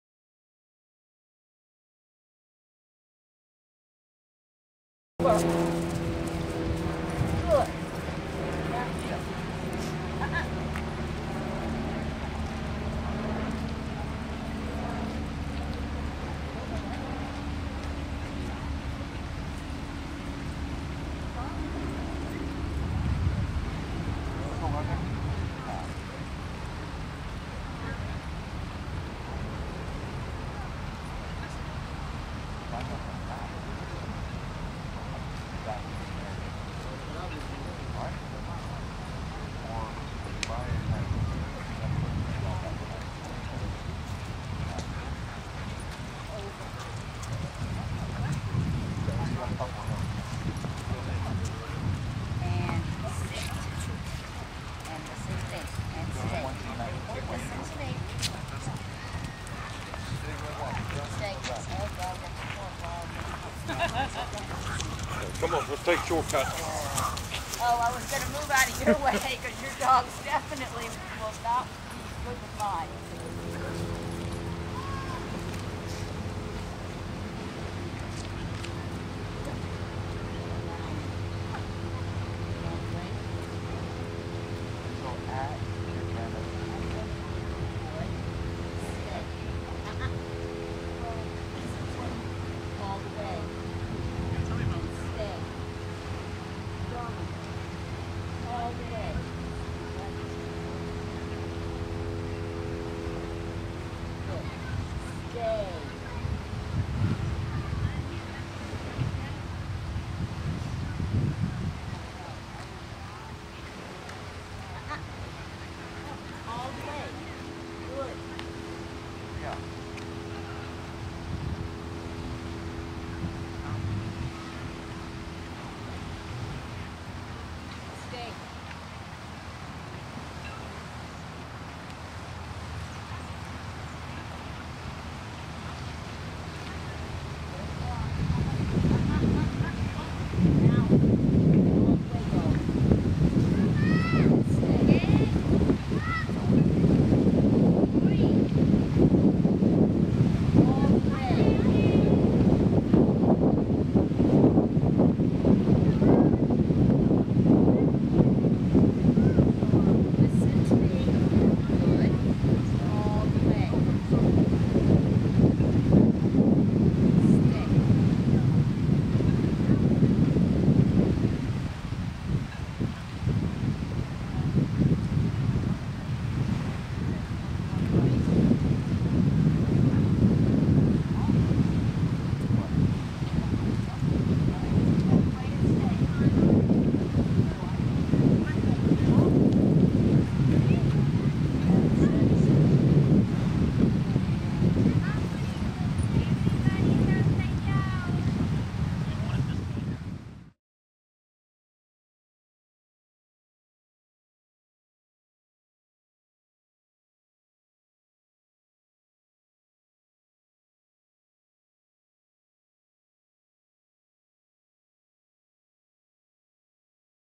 Dog Training Heavy Distractions Out of Control Dogs.wmv
Dog Training, Heavy Distractions, and Example of Dogs NOT being walked well in a public place!  It's tough out there training in public places, but it's a great way to proof your dog's behavior!